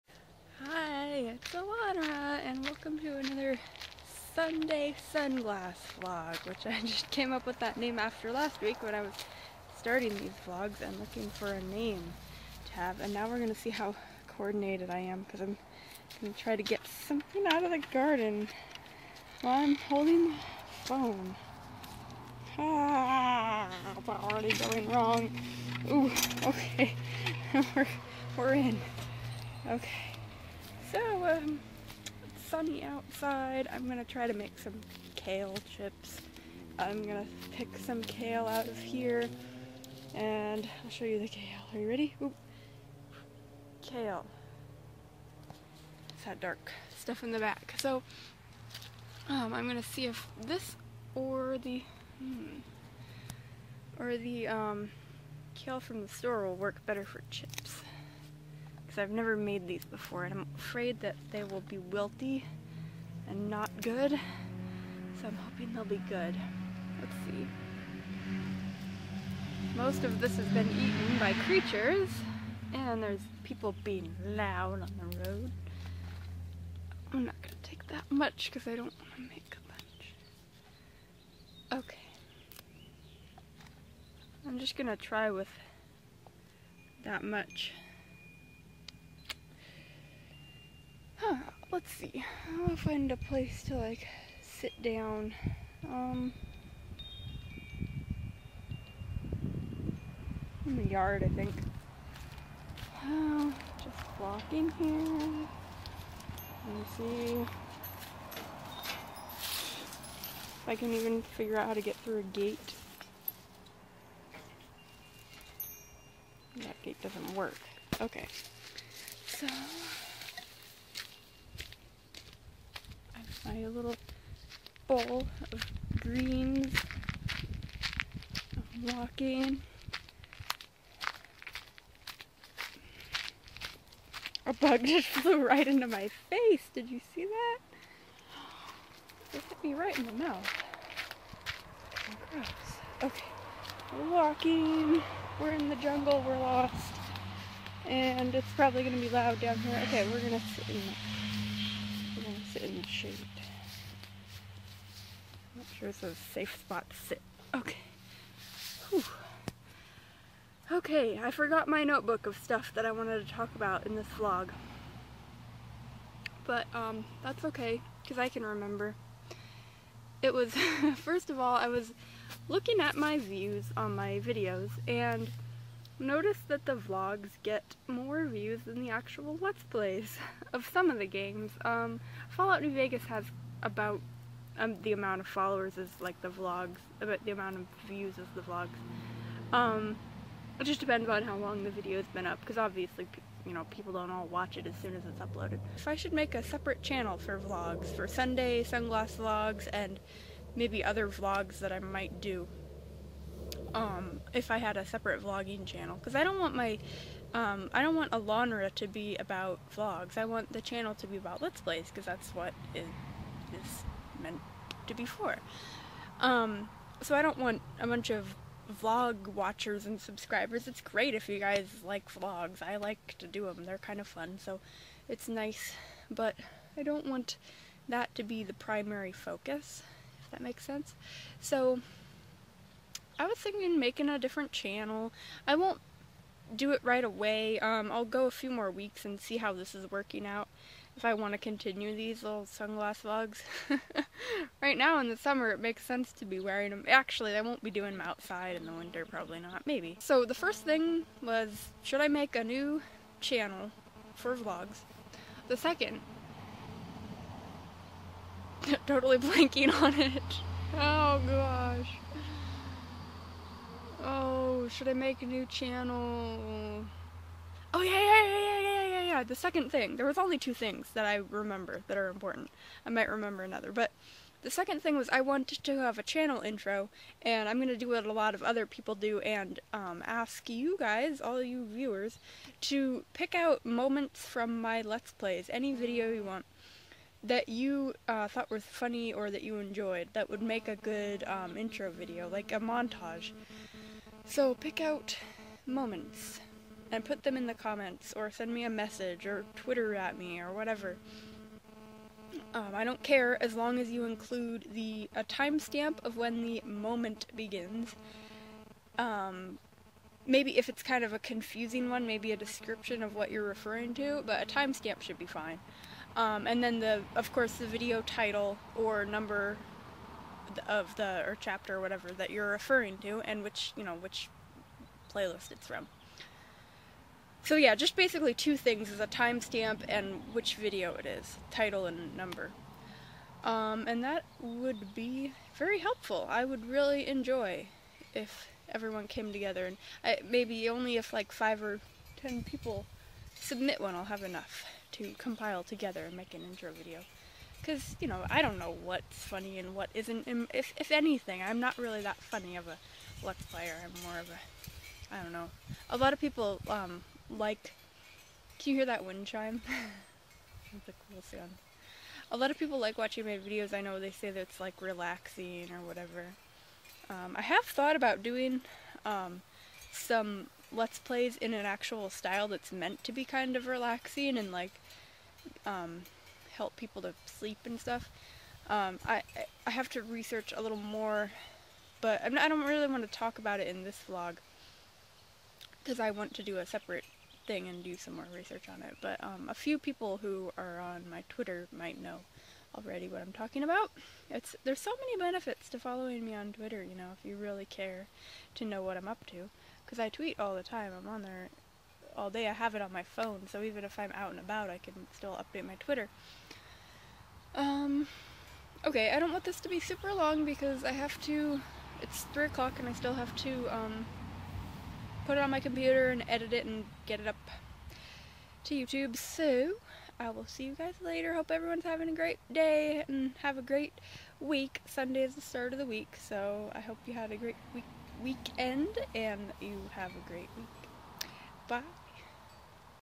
Sunday Sunglasses Vlog #2 - Tell Me Your Favorite Moments!
Please let me know what you think. Submit your favorite moments from any of my let's play videos :)
Remember to include a time stamp and the video title or number!

Thanks!!